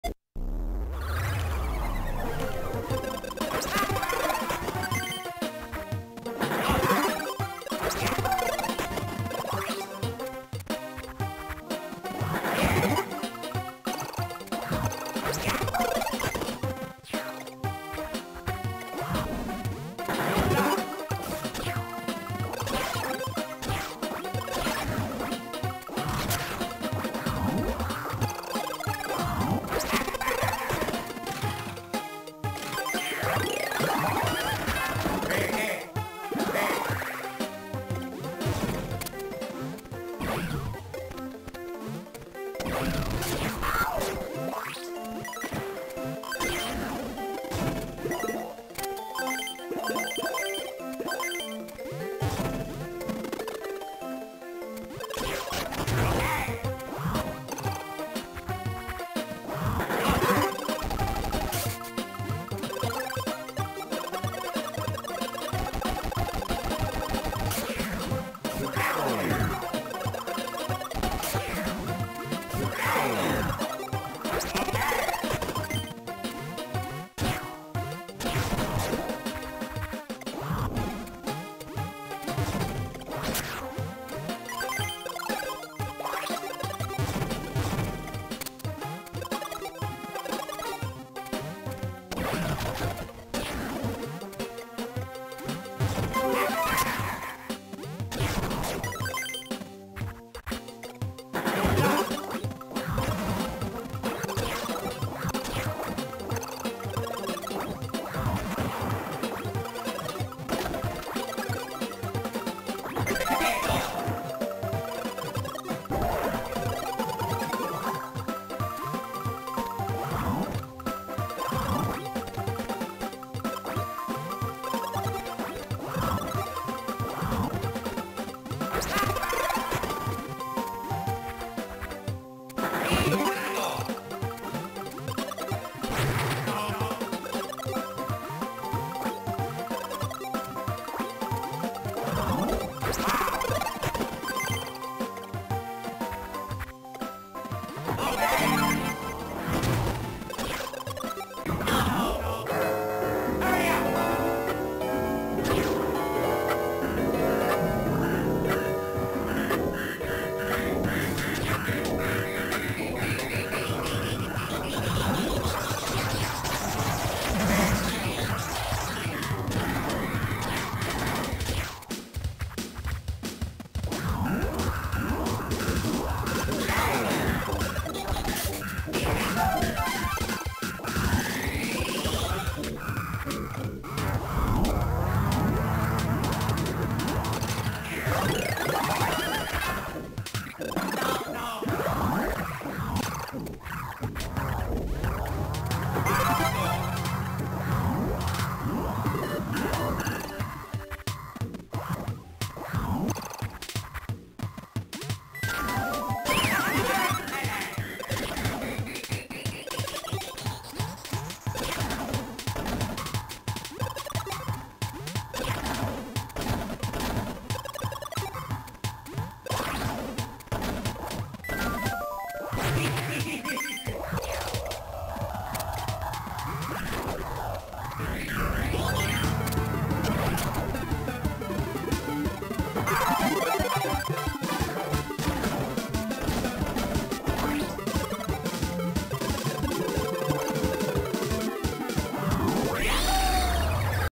Domino Row 100% (Normal) Zipless demonstration
The final chest room can be done in a more risky fashion. Additionally, the purple pipe with the enemies falling into water is not necessary; instead you may get more coins in the water domino room and the next spike domino room.